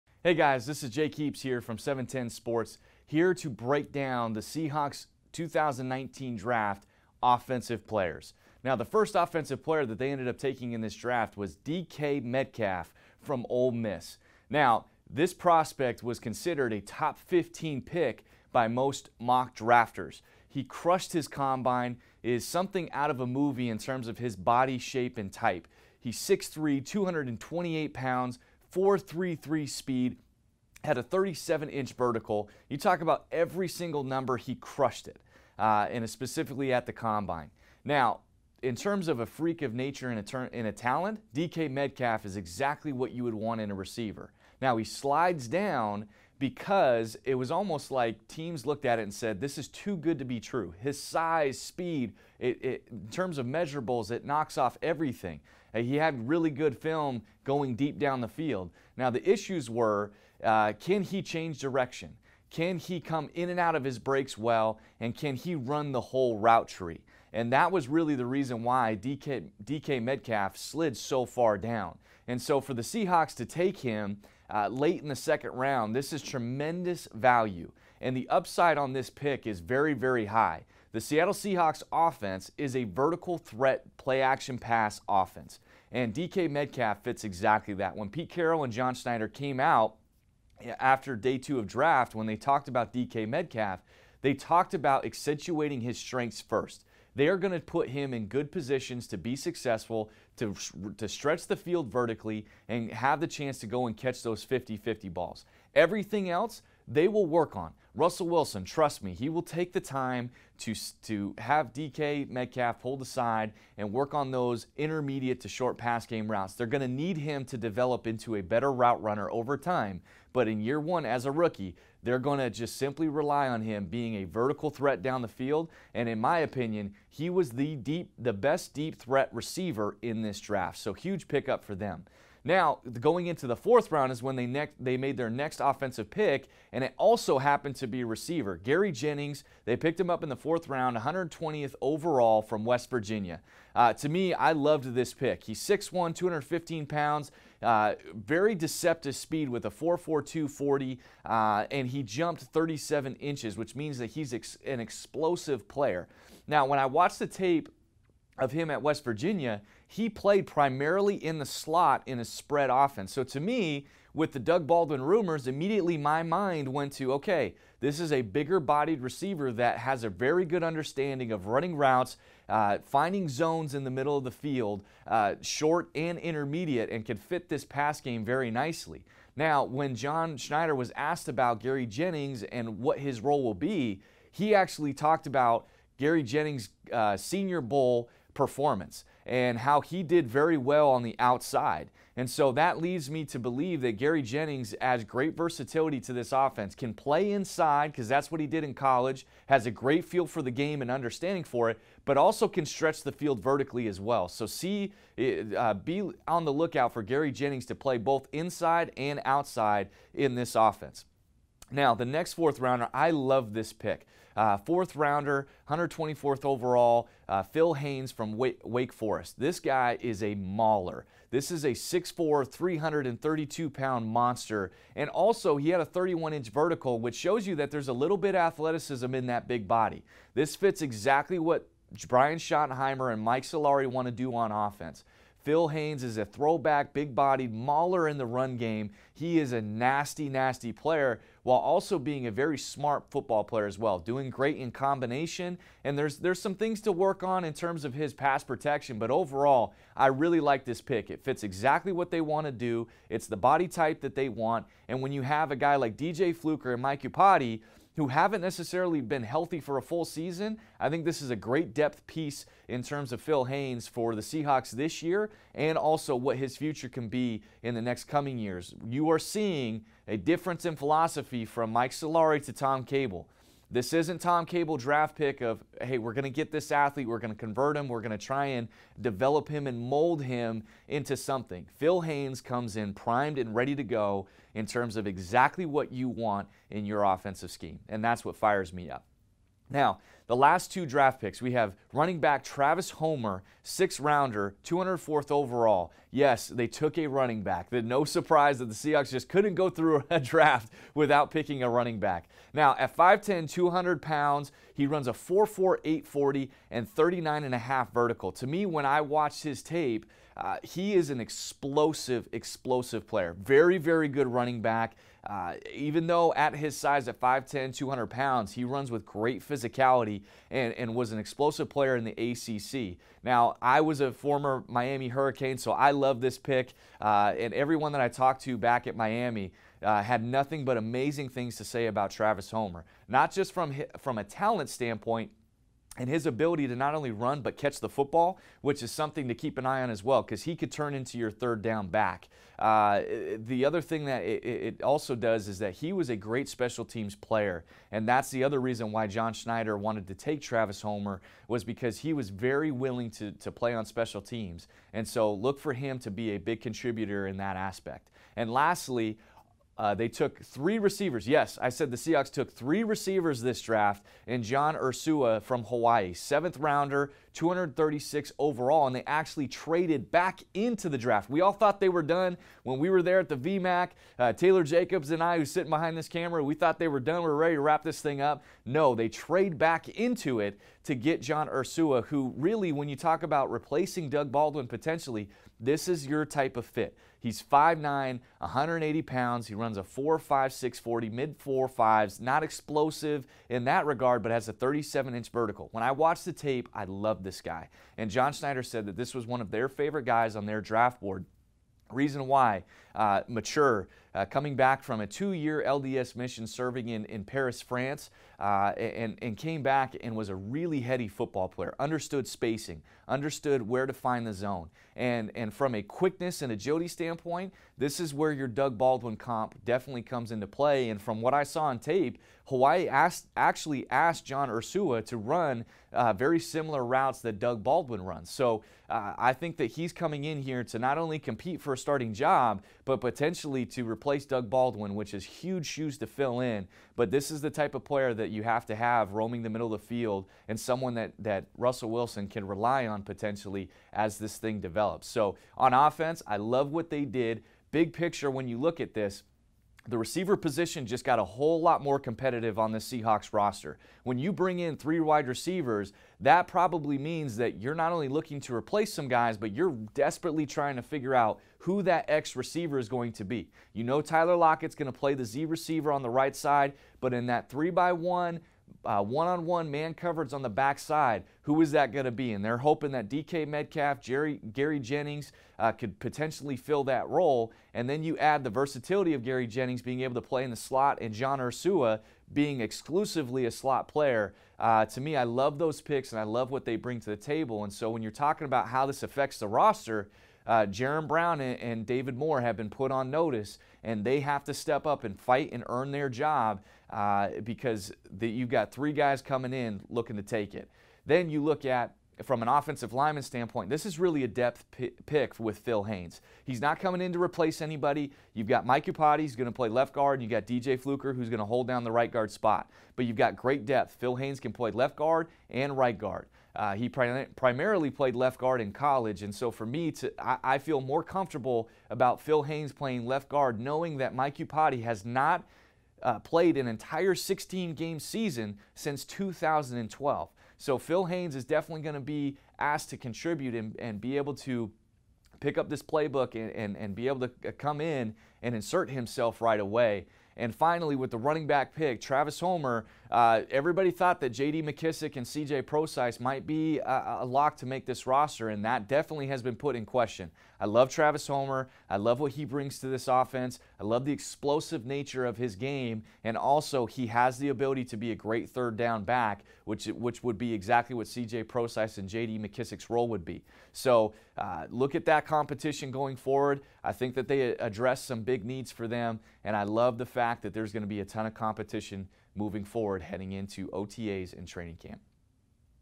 Jake Heaps' Seahawks Draft Breakdown: Offense
710Sports.com's Jake Heaps breaks down the Seahawks Draft. First, he breaks down all the offensive players. What will these players look like in the Seahawks system? How will they fit in Seattle. Watch above to find out now.